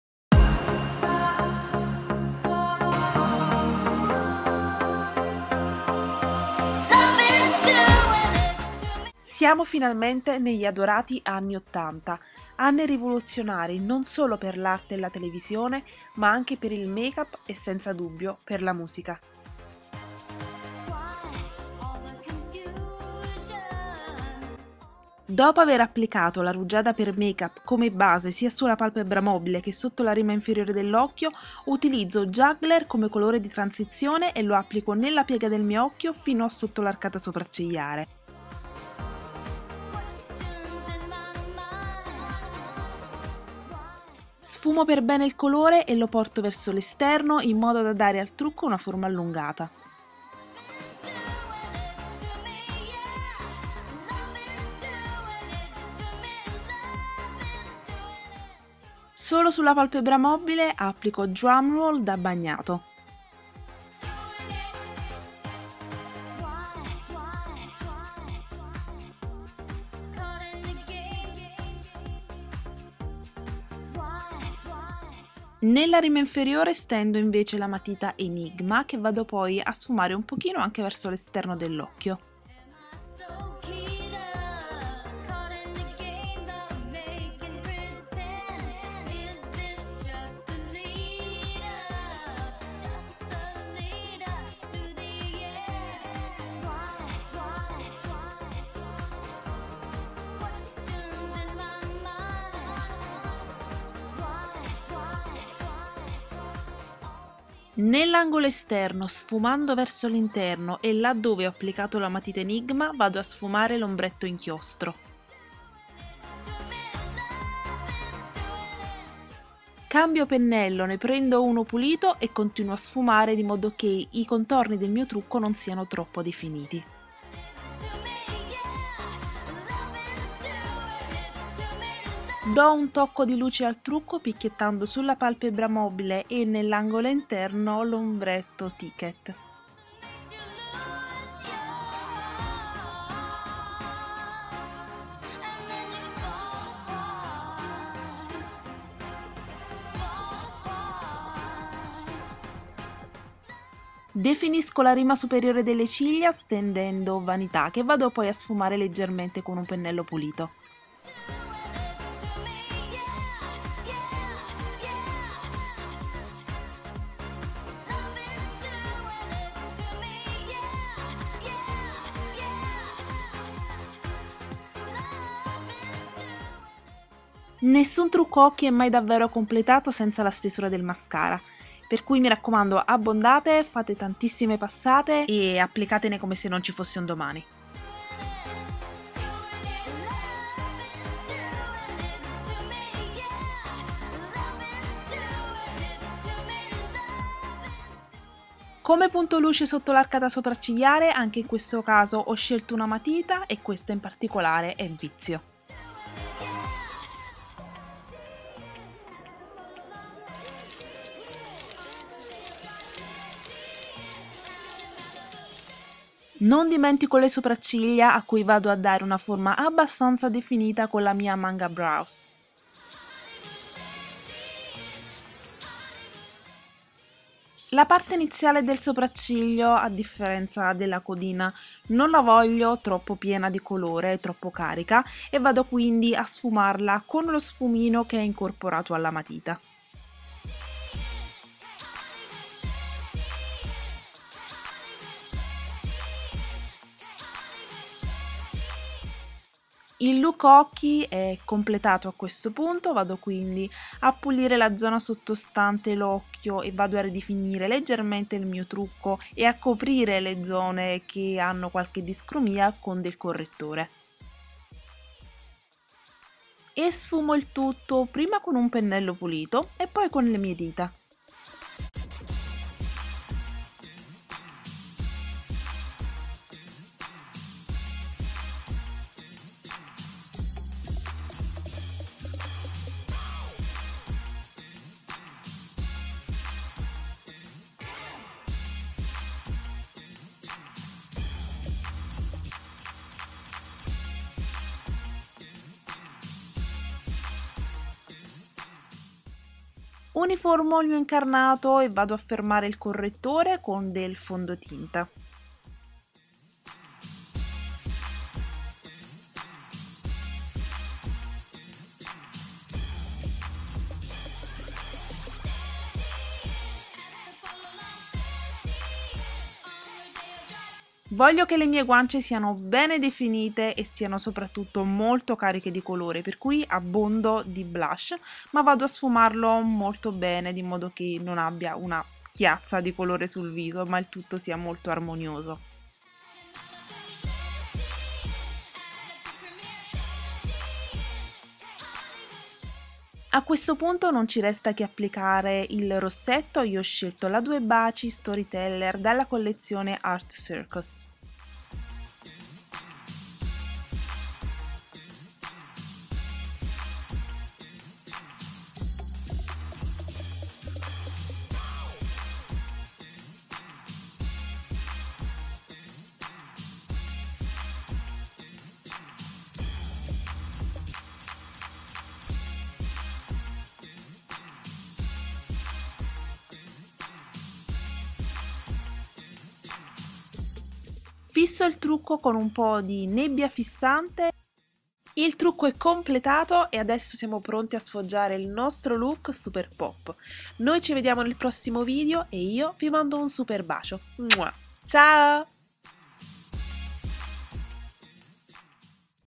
POP ANNI '80 makeup tutorial (ITA)
Un salto nel passato!
I favolosi anni '80 e la musica POP hanno ispirato il nuovo tutorial di Brunella.
Un trucco scintillante ed estroso per chi non ha paura di osare con il colore!

PRODOTTI USATI:

- Rugiada per makeup
- Ombretto minerale Juggler
- Ombretto minerale Drumroll
- Pastello occhi Enigma
- Ombretto in cialda Inchiostro
- Ombretto minerale Ticket
- Pastello occhi Vanità
- Mascara Occhioni
- Pastello occhi Vizio
- Matita per sopracciglia Manga Brows
- Correttore Nascondino
- Fondotinta Flat Perfection
- Blush Acrobat
- DueBaci nella tonalità Storyteller
- Nebbia Fissante